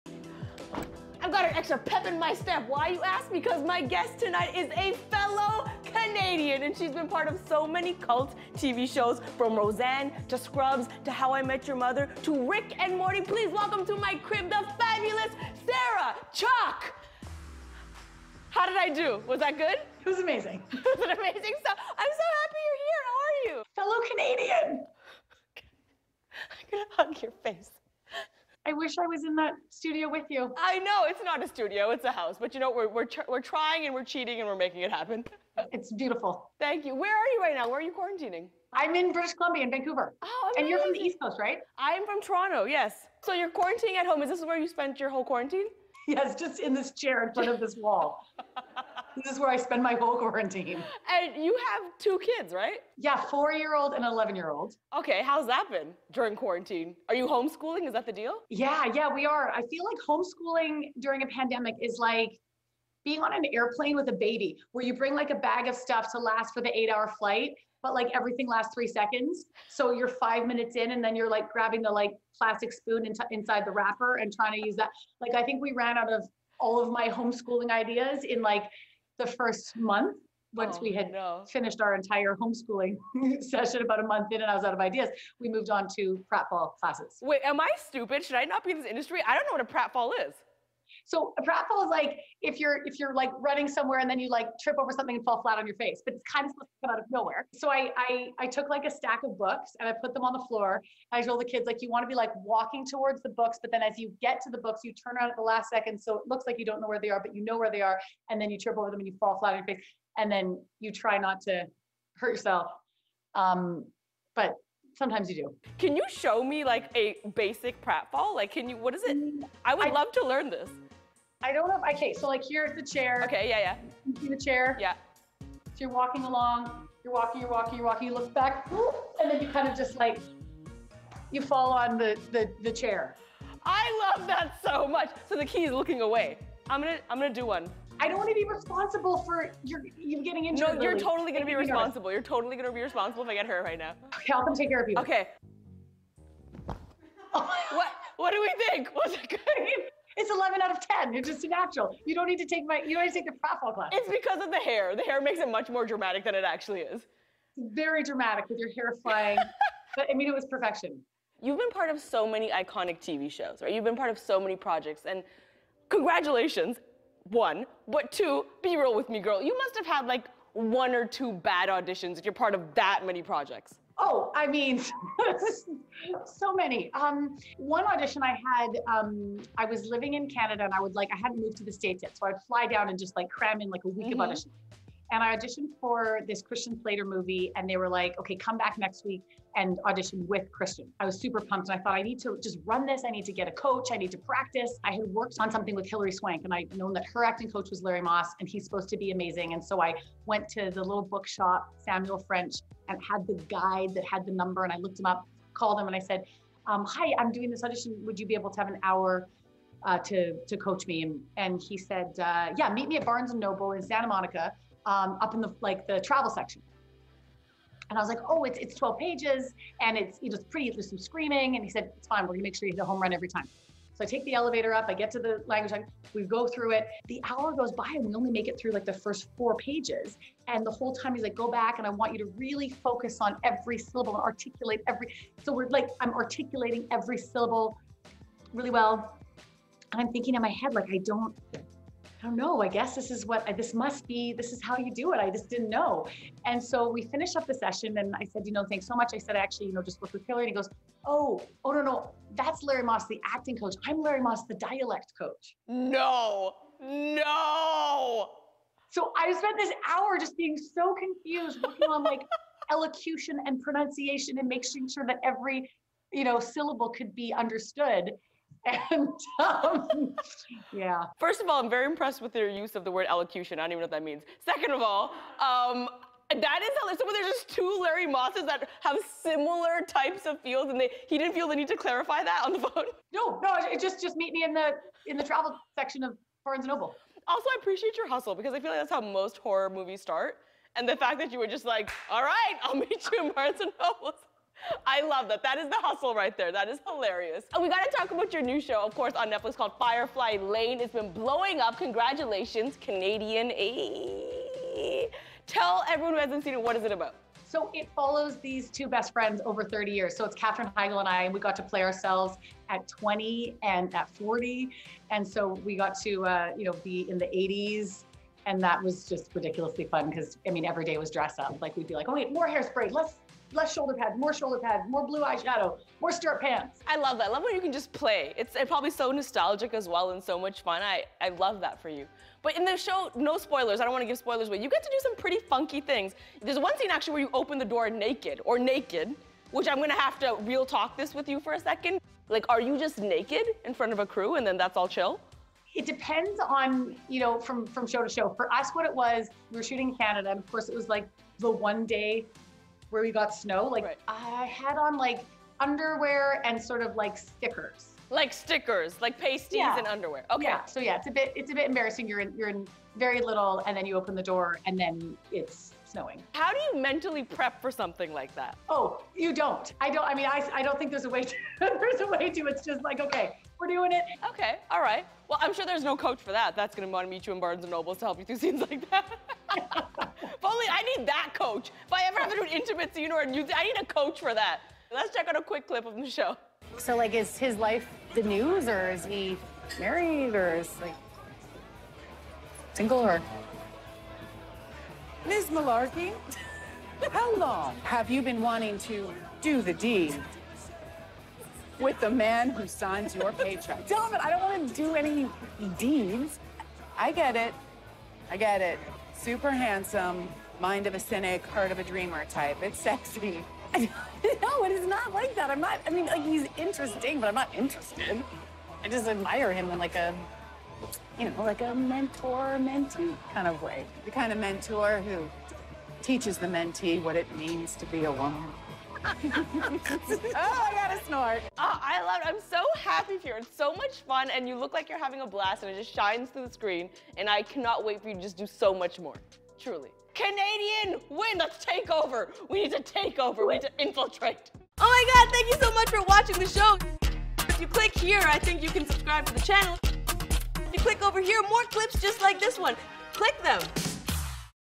Sarah Chalke’s Nude Scene in Firefly Lane Was Embarrassing
Sarah Chalke shows Lilly how to pratfall, talks about trying to meet with Hillary Swank’s acting coach in her early career and reveals there is no way to mentally prepare for a nude scene.